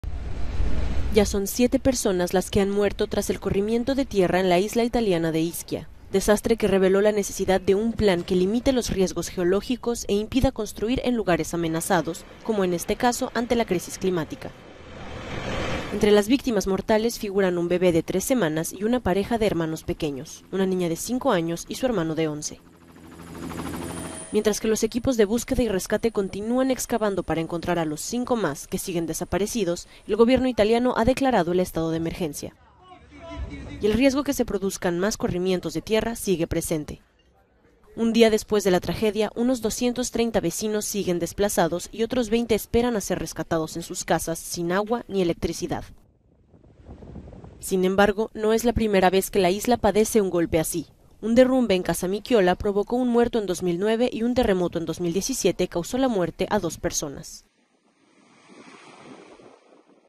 El corrimiento de tierra de Ischia que dejó 7 muertos recuerda la fragilidad de Italia
El desastre dejó 7 muertos, y reveló la necesidad de un plan que limite los riesgos geológicos e impida construir en lugares amenazados, como en este caso, ante la crisis climática.